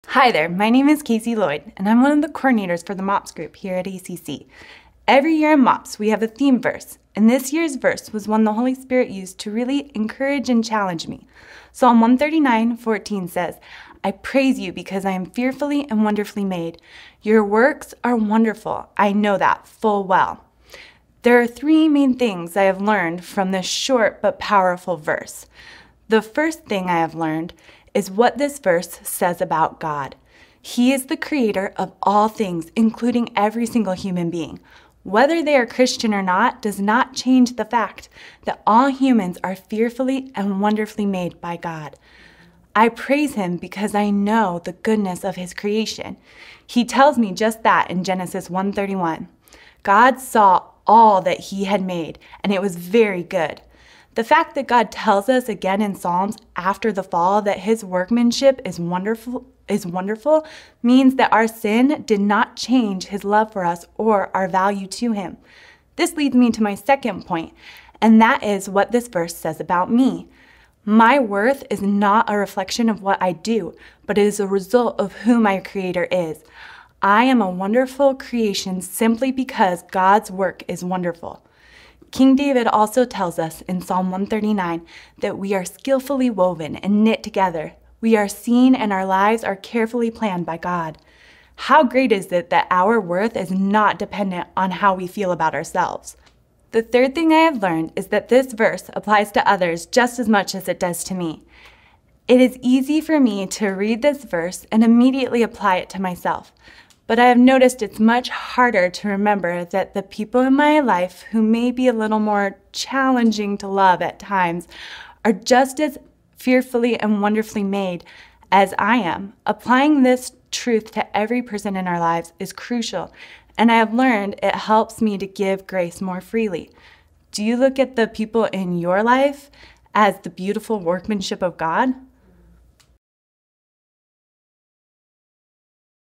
Daily Devotional 2-3-21
It's easy to remember that we are wonderfully made by God. It can be challenging, however to remember that about others | Arizona Community Church | AZCC.org